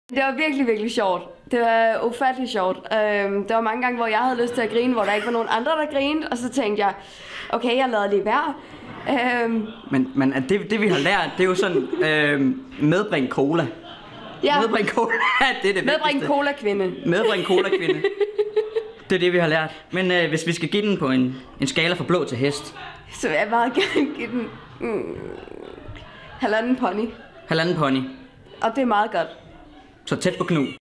Sjit Happens 2 - En halv pony
Vi inviterede en flok fans til forpremiere på den nye sæson, her kan du se hvad de synes om den.